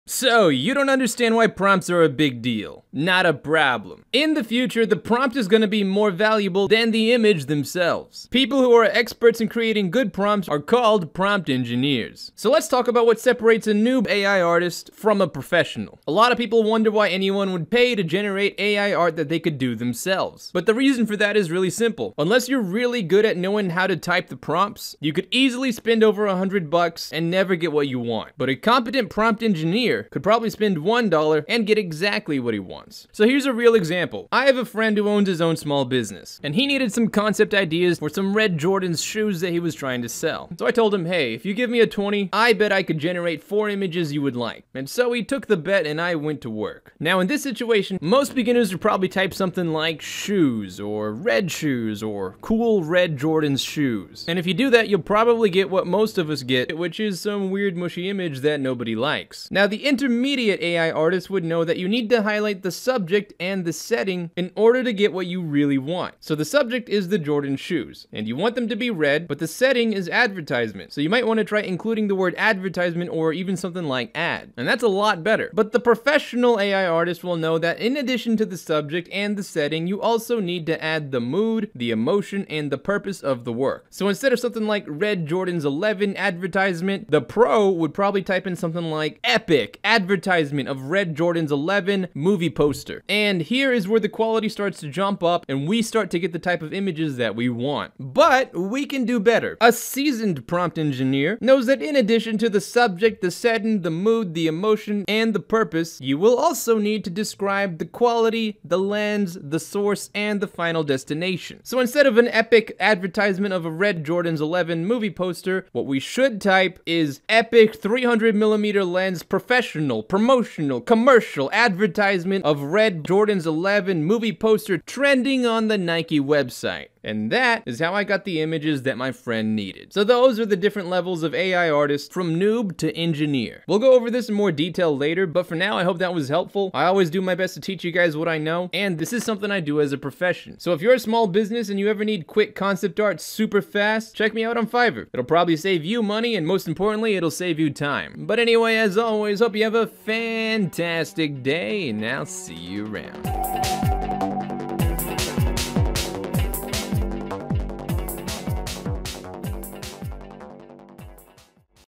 Dall-E - How To PROMPT Like A PRO (In 3 Minutes!!!)
Alright, now that you know how Dall-E works, here's a quick example of how to start typing your Prompts like a Professional-!!